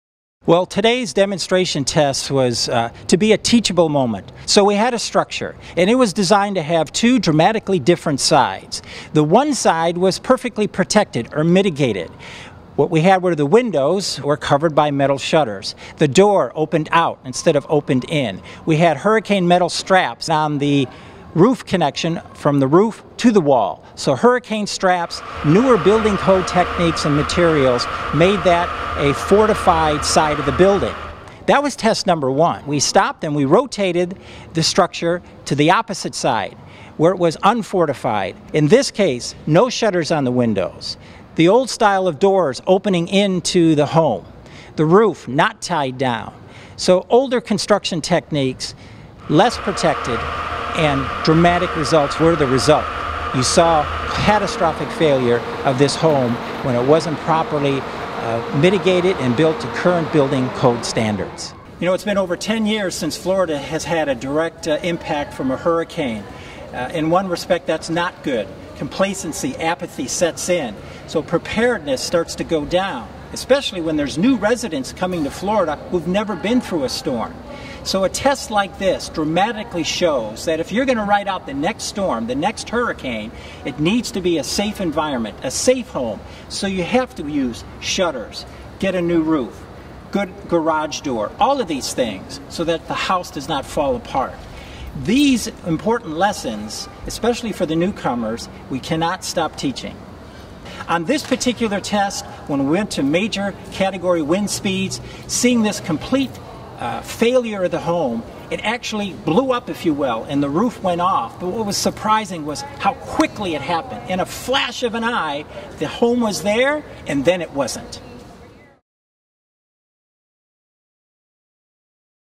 VIDEO: FIU's Wall of Wind Demonstration
07/27/16 - FIU ‘s Erik Salna and team demonstrated what hurricane force winds can do to both a fortified and unfortified house at the Engineering Center’s Wall of Wind in Miami on Wednesday, July 27, 2016.